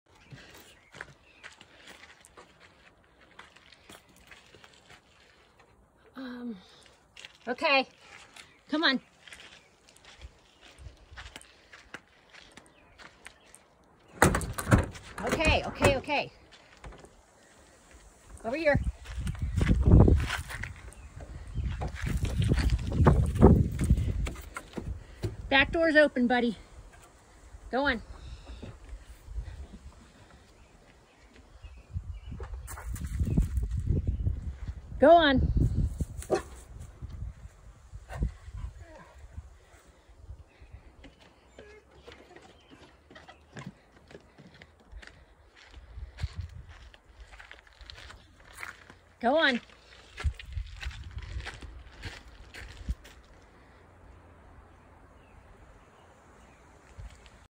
Trapped Bear Freed from Car || ViralHog
Occurred on June 30, 2020 / Como, Colorado, USA

"For the second time in 2 months, a bear got into my car and the door closed behind it. The door was unlocked and I had been cleaning the interior, and went indoors to make a phone call. I just got off the phone, when I heard my car horn honking. I looked out the window and there was a bear sitting in the driver's seat, honking the horn as if to say, 'Get me out of here!' I went outside and decided the easiest way to let him out was through the back hatch. I opened it and let him know he could exit through the back. He seemed to appreciate getting out quickly, and turned around to say thank you!"